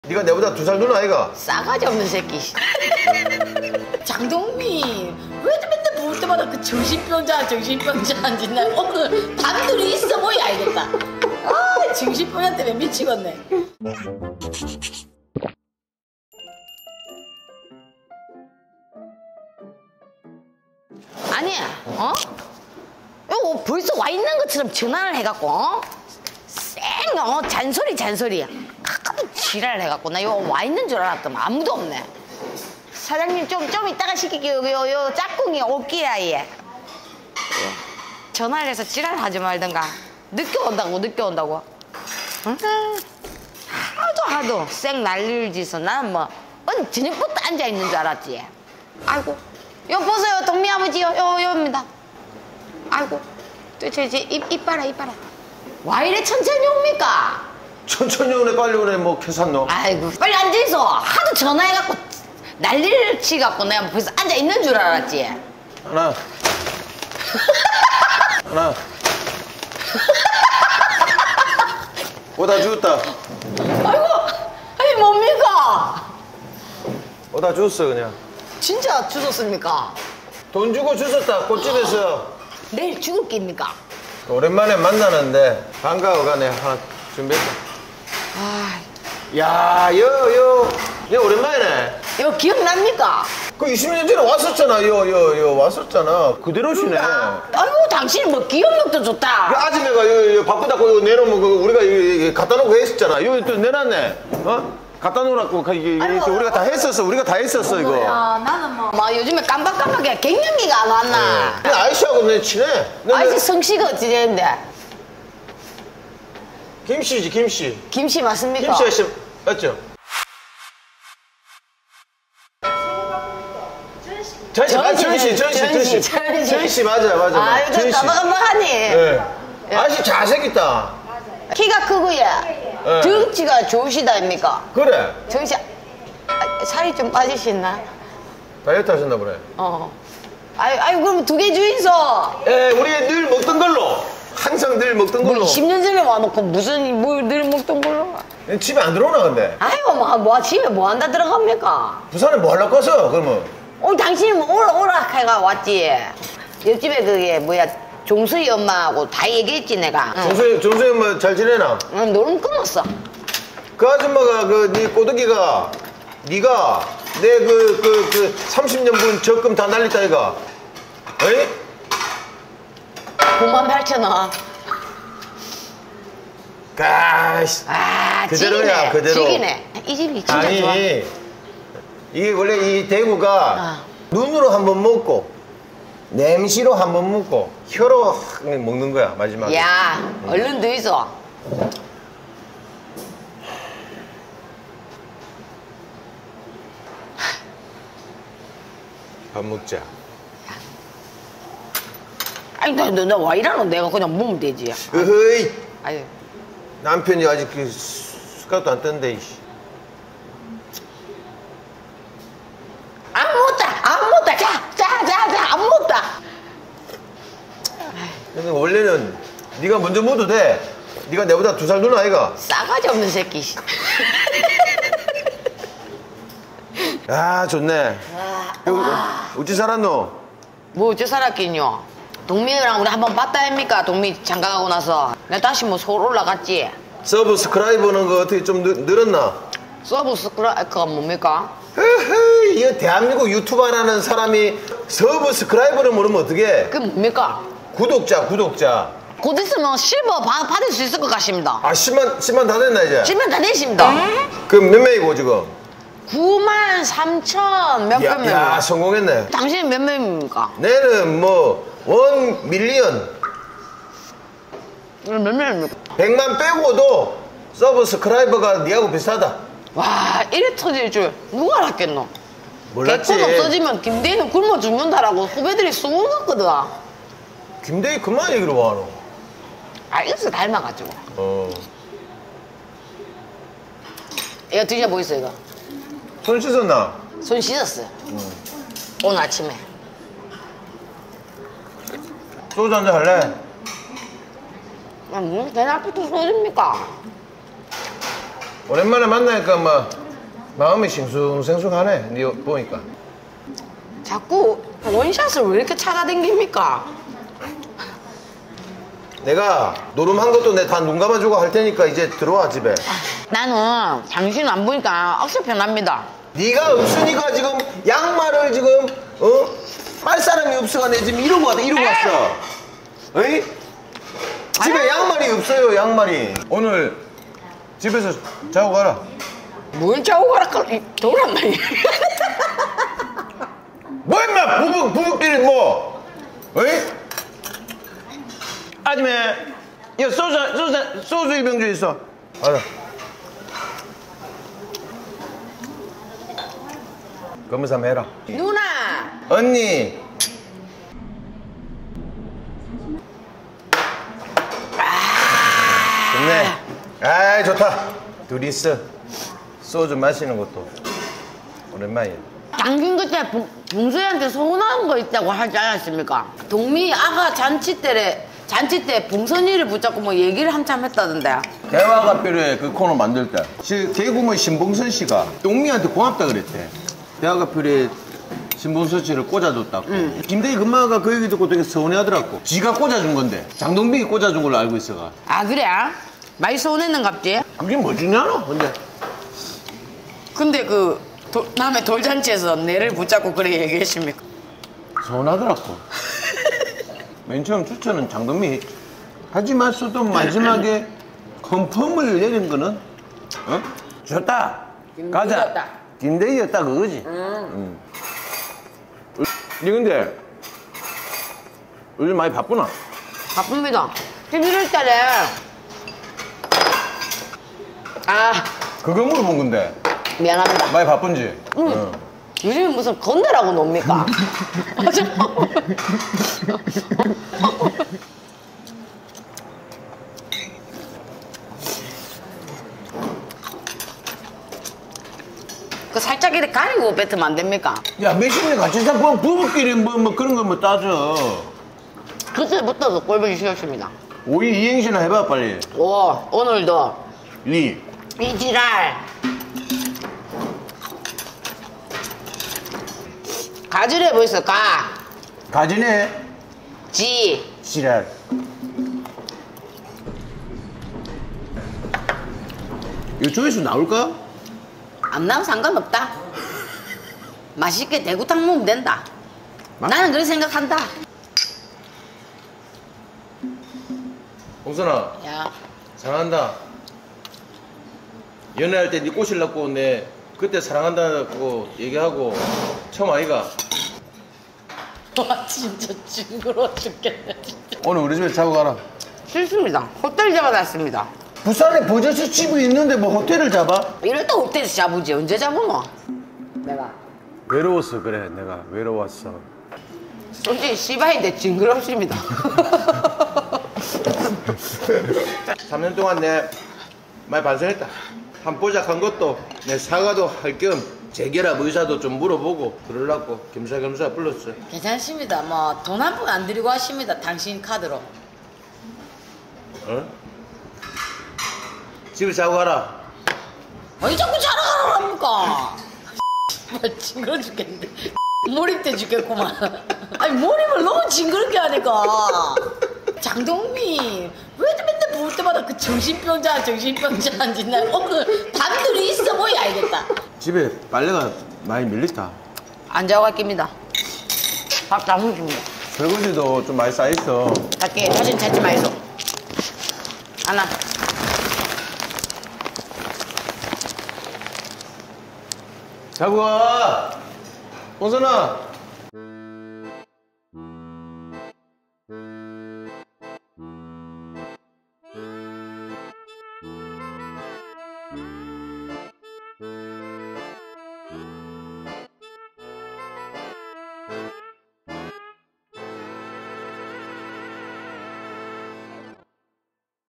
🍺술묵자🍺 꼰대희가 동민 엄마를 부른 이유🫢
20년 전 데이트 장소에서 다시 만난 두사람

"오랜만에 만나니까 마음이 싱숭생숭하네..."
추억에 잠긴 꼰대희의 플러팅에 동민 엄마의 반응은?🤭
※ 본 콘텐츠 및 출연자와 무관한 댓글과 무분별한 욕설, 비난 등의 악의적인 댓글은 무통보 삭제될 수 있습니다.